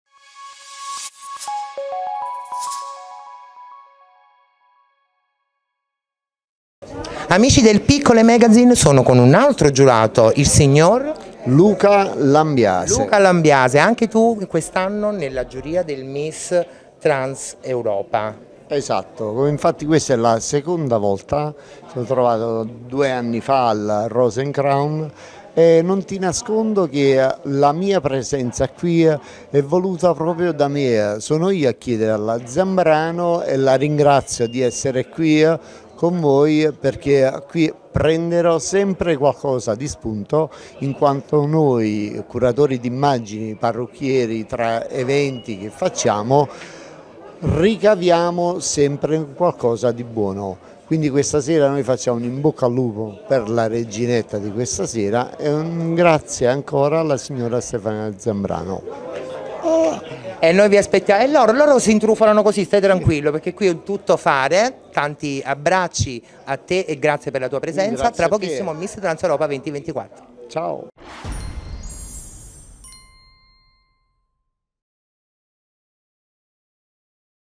Intervista a Luca Lambiase alla finale del Miss Trans Europa 2024
Ecco l’intervista a Luca Lambiase alla finale del Miss Trans Europa 2024, che si è svolto nei giorni 8 e 9 Settembre 2024, al Teatro Troisi in via Giacomo Leopardi 192, 80125 Napoli (NA)